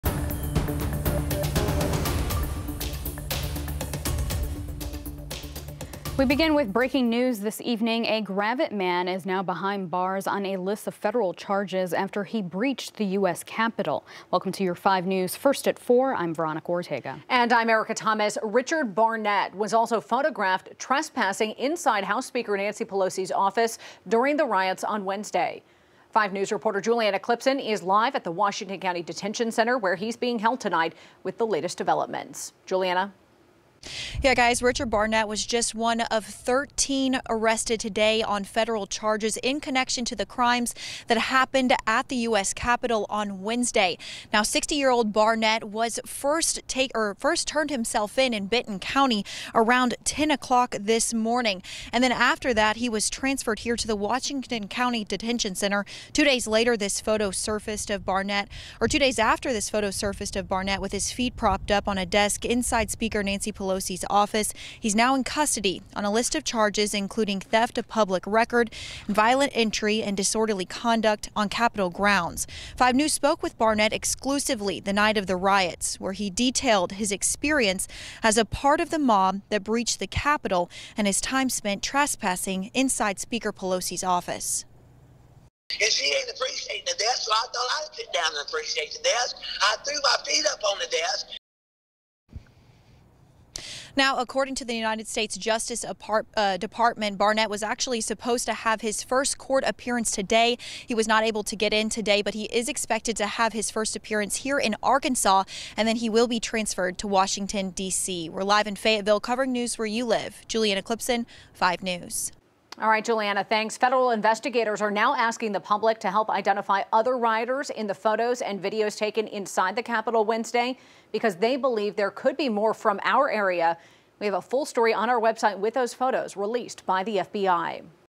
Extremist spotted in Nancy Pelosi's office during Capitol riot arrested in Arkansas
Richard Barnett, 60, of Gravette, Ark. turned himself in Friday morning.